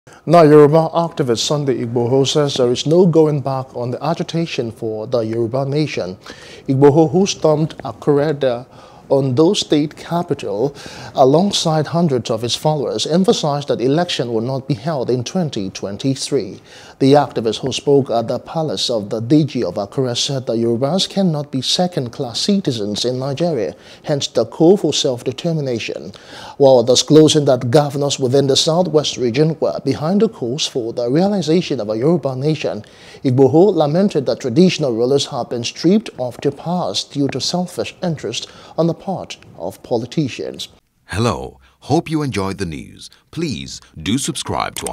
Oduduwa Republic: Igboho Storms Ondo, Says No Going Back On Yoruba Nation | NEWS
Yoruba activist, Sunday Igboho says that there is no going back on the agitation for Yoruba nation.

Igboho, who stormed Akure, the Ondo State capital alongside hundreds of his followers, emphasized that election will not be held in 2023.